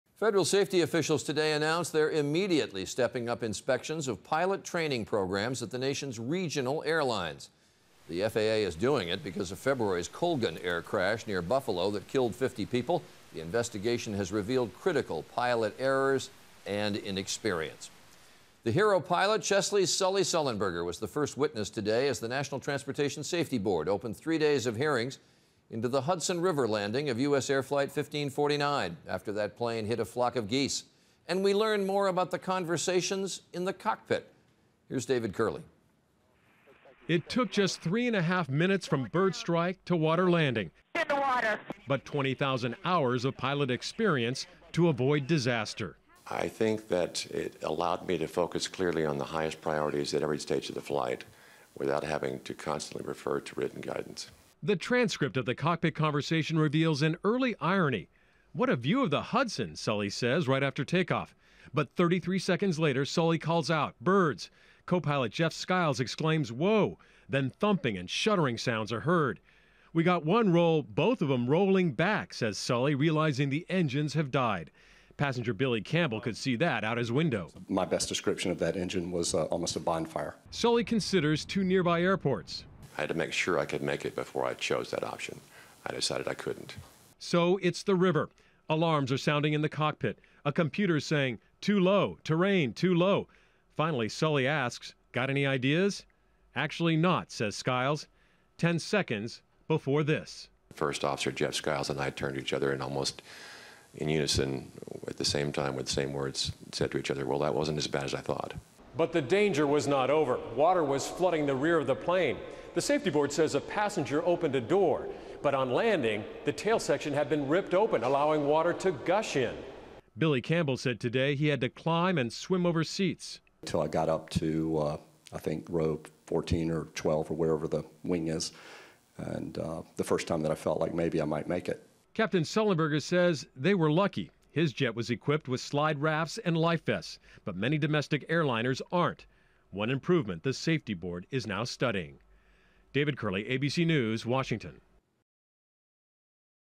Hudson 'Miracle' Capt.: We Were Lucky
NTSB hearing and cockpit tapes underscore the importance of flight experience.